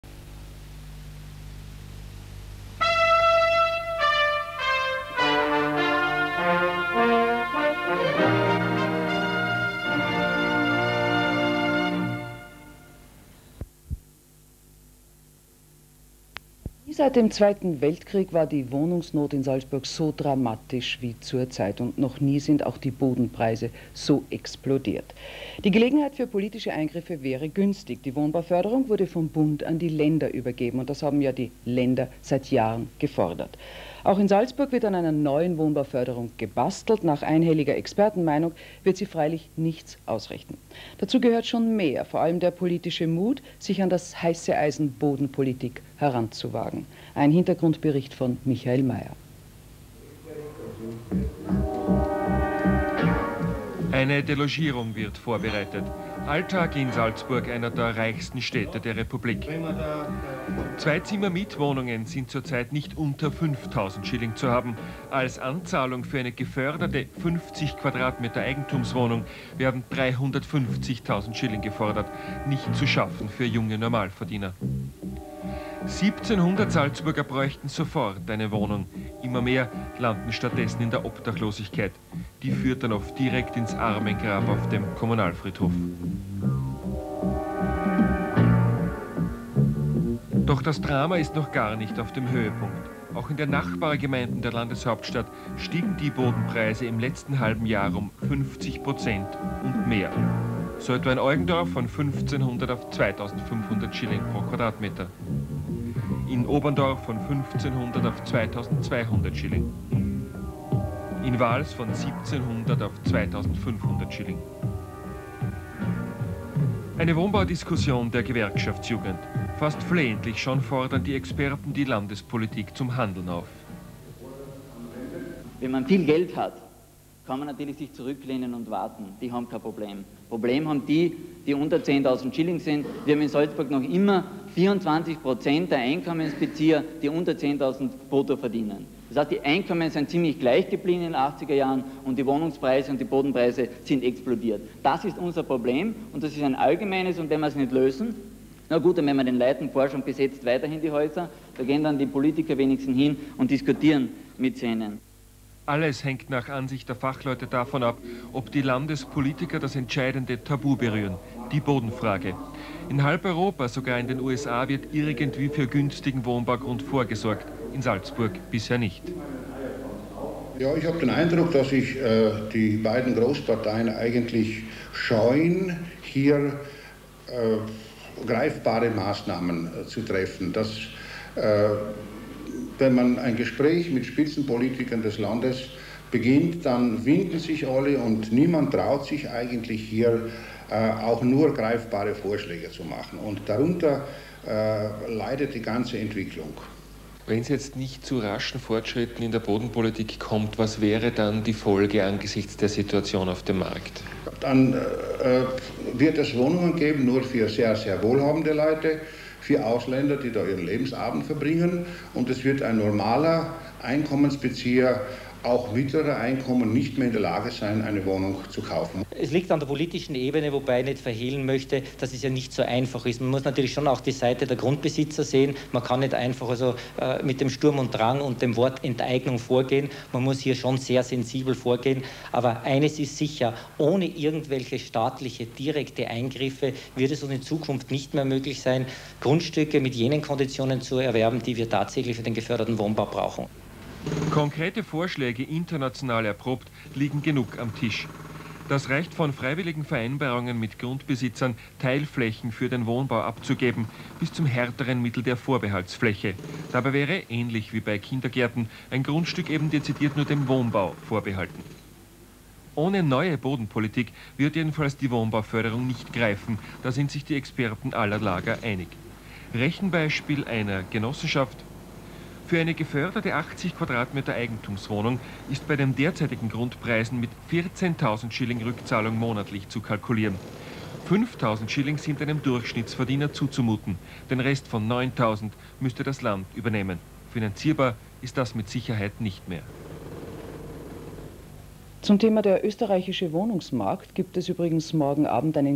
1990 - ORF Salzburg - ÖGJ Salzburg TV Beitrag zur Wohnsituation in Salzburg
Aktion der ÖGJ Salzburg zum wohnen in Salzburg im Jahre 1990
c Herbert Huber, RS GPA-djp Salzburg